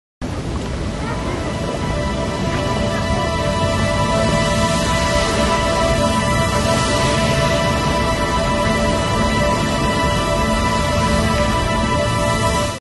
teflon christian blau 2004, art
teflon
is a comment on the threats
that are artificially produced
through media, used not only
by filmmakers but more by
politicans
2004

exhibited at:
scope art fair
at hotel "the standard"
8300 Sunset Boulevard